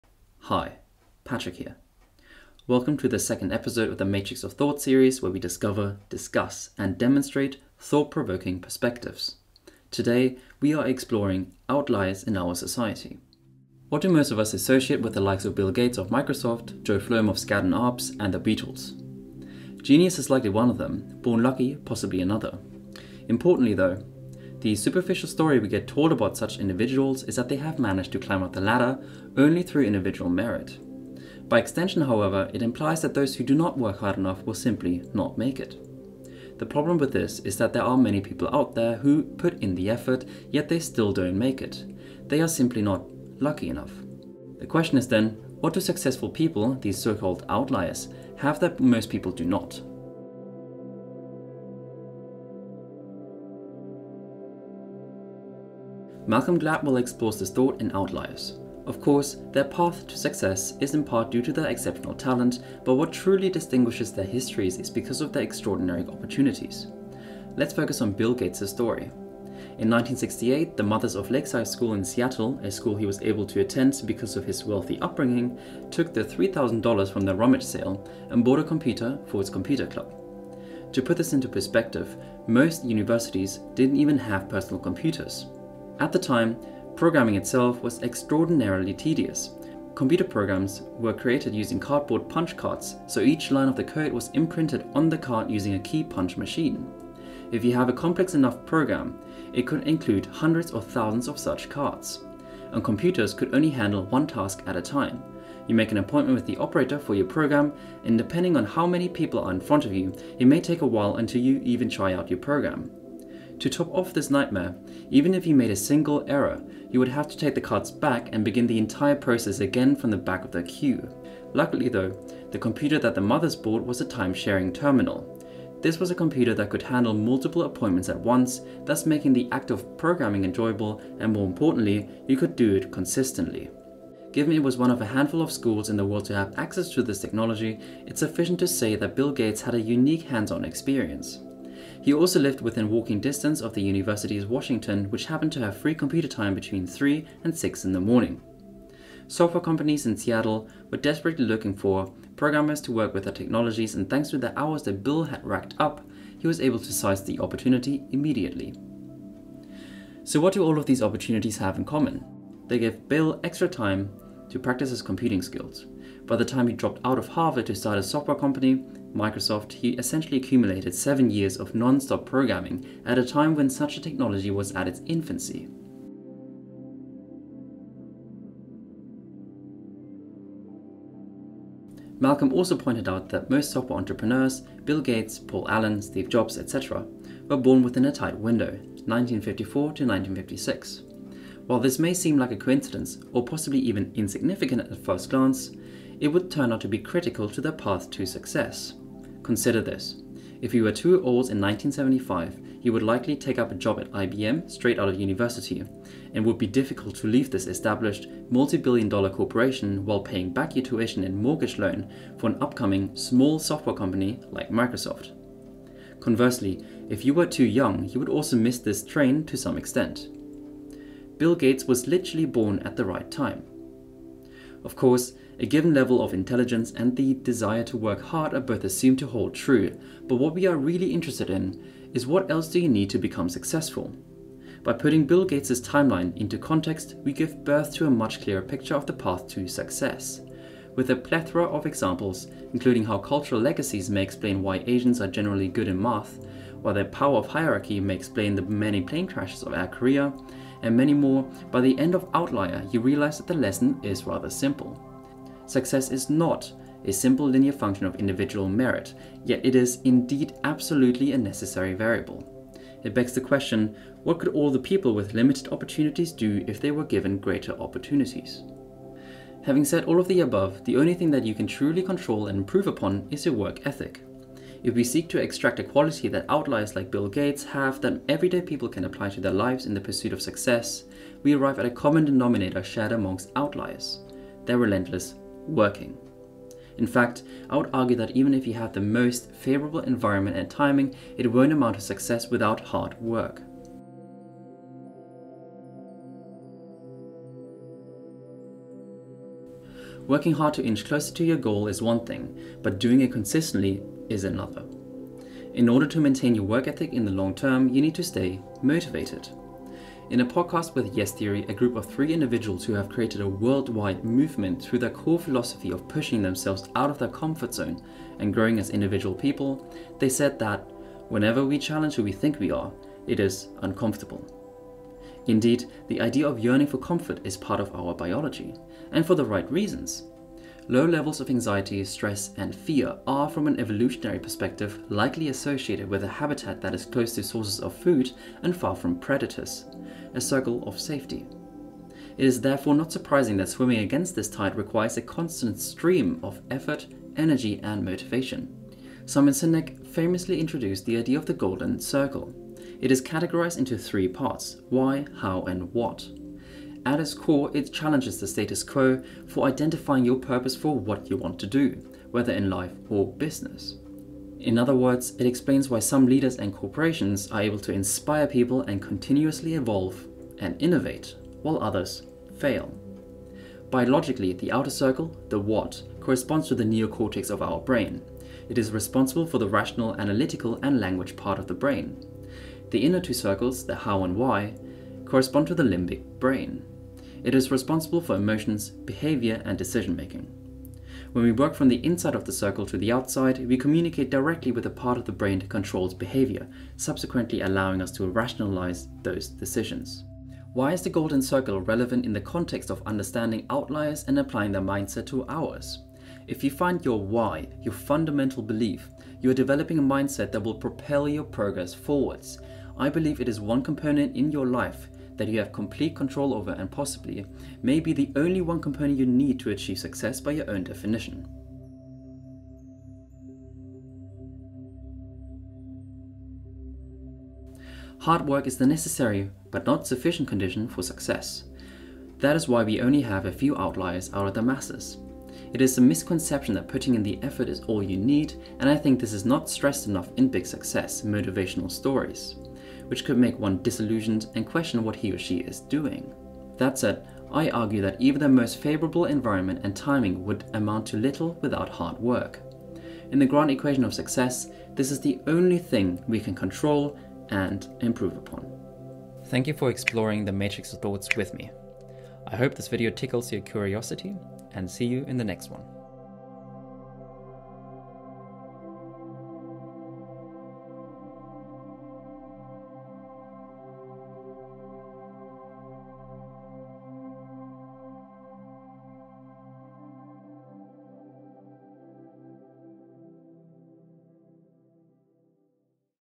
What's Different About Bill Gates?
How do outliers in our society achieve great success and what can we do to also be like them?

Welcome to the second episode of the Matrix of Thoughts series where we discover, discuss and demonstrate thought-provoking perspectives.

Chapters
0:00 - Introduction
0:50 - Case Study: Bill Gates, your environment and background matter
2:57 - Your timing also matters
4:55 - Motivation is the fuel to keep your hard work running
7:02 - Conclusion